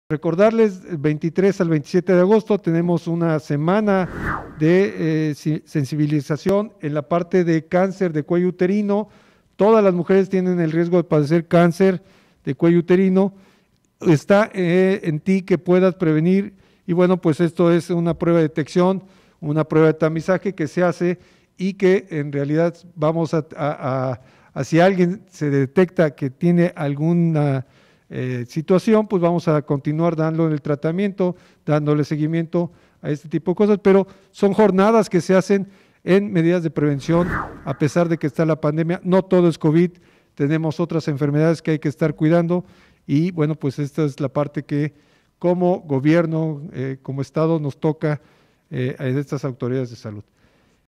Semana de la prevención del cáncer del cuello uterino
Invitan a todas las mujeres mayores de 18 años para realizarse la prueba que detecta el cáncer del cuello uterino.